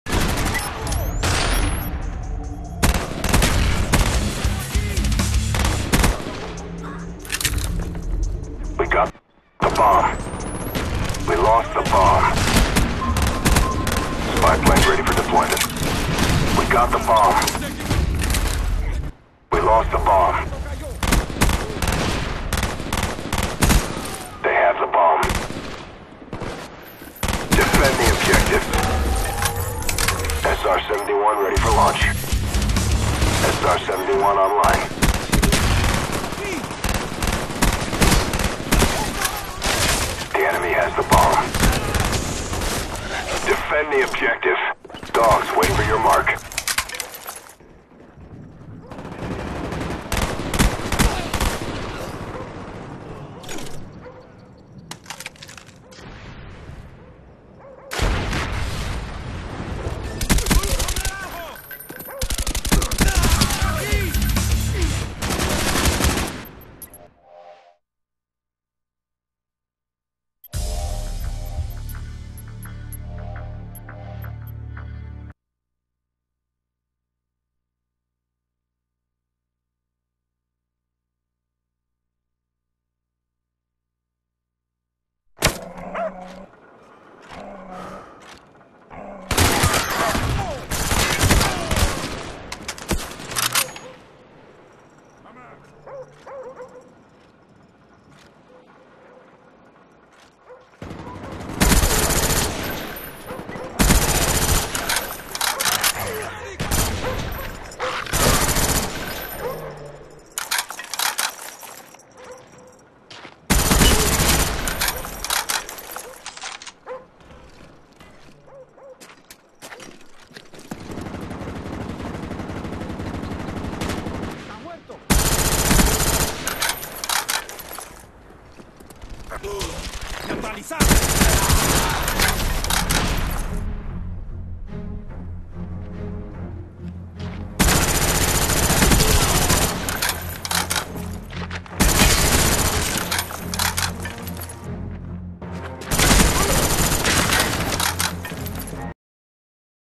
Best Streak Ever : G11 | Ep. 1
26 Killstreak right in the beggining of a sabotage game. 10 likes for next episode!